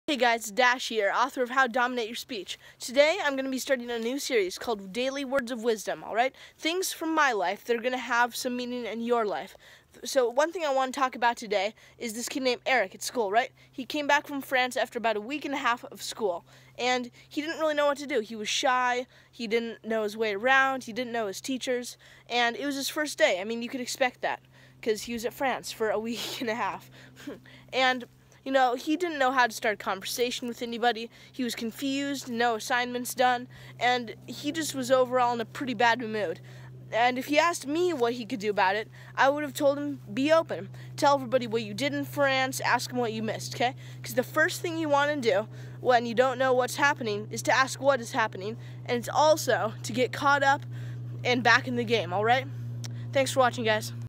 Daily Words Of Wisdom- Episode one
These are my daily video series, all of them with a message and a purpose. For this one, it's never to forget your purpose.